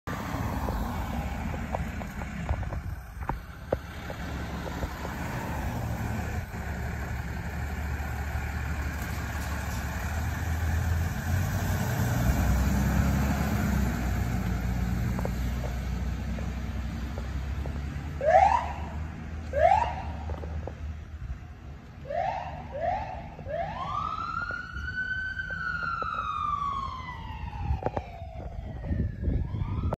LAFD Rescue 85 responding
LAFD  Rescue 85 responding to another medical emergency
RESCUE 85 IS A DODGE RAM